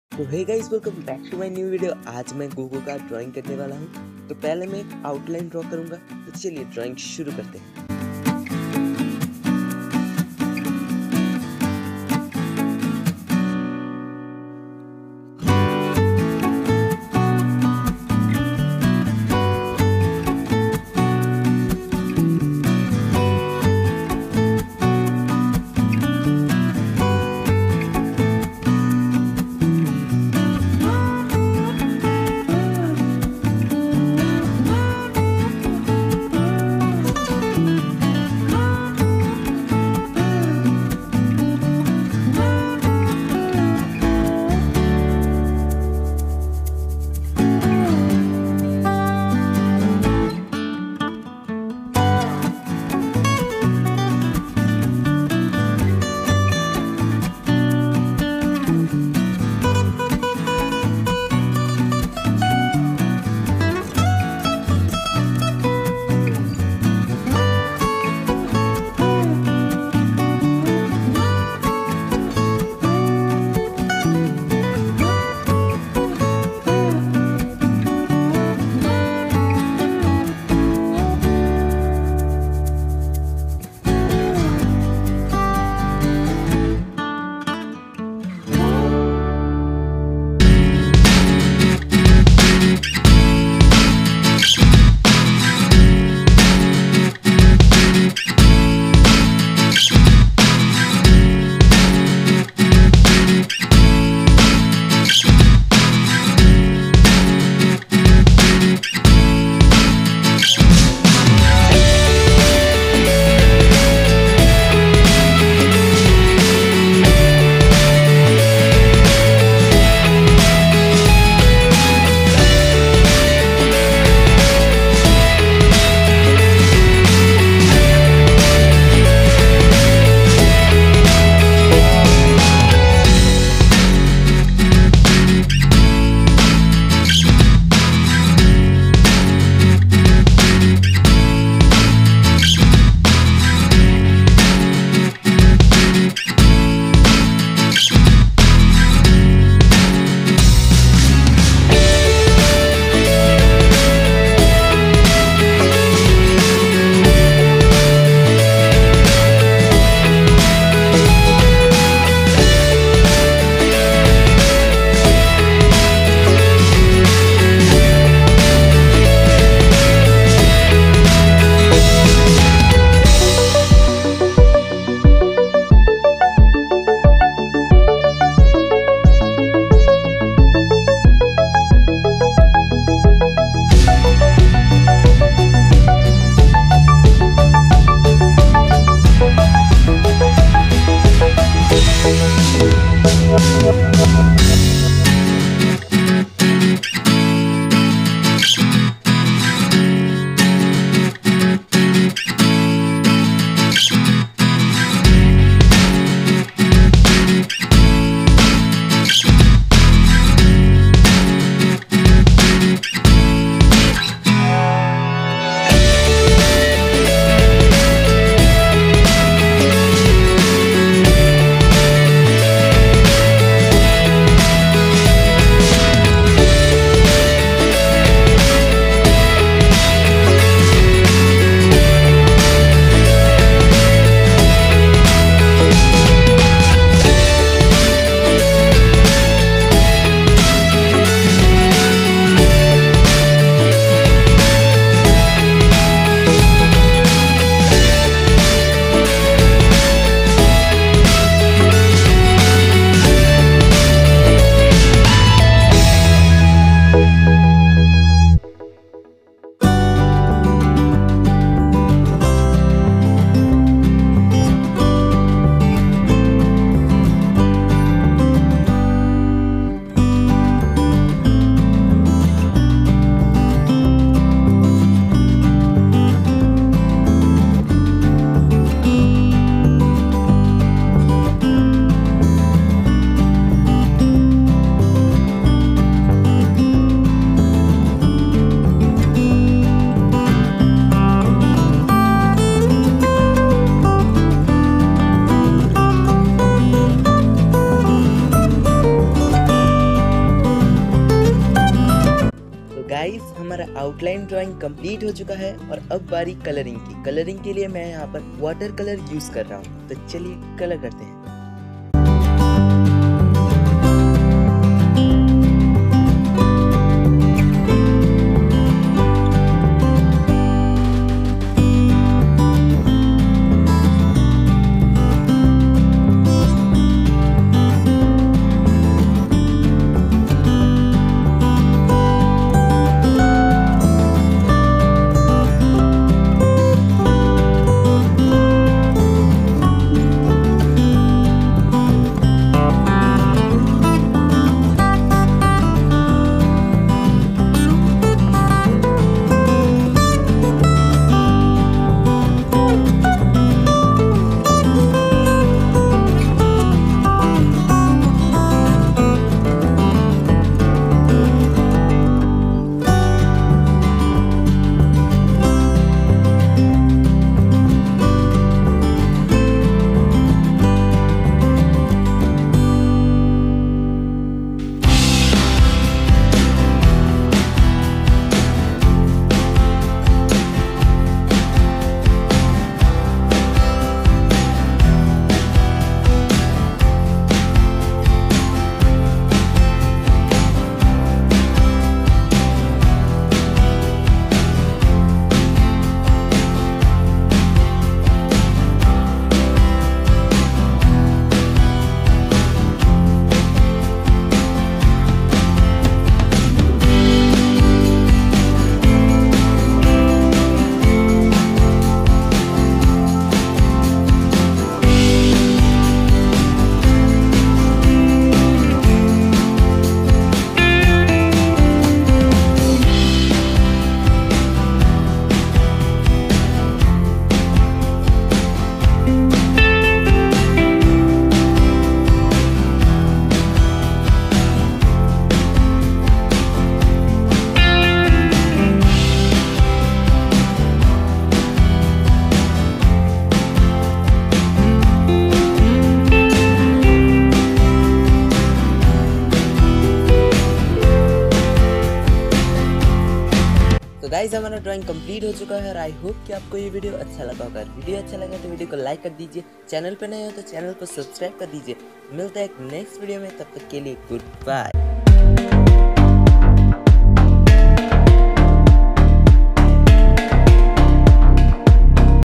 Goku Drawing Easy || Goku Drawing With Water Colour || Goku Drawing Step By Step
In this video I am drawing Goku with water colour 💖

Used things:-
Paper - sketch book paper
Pencil - 6B pencil
Colour - water colour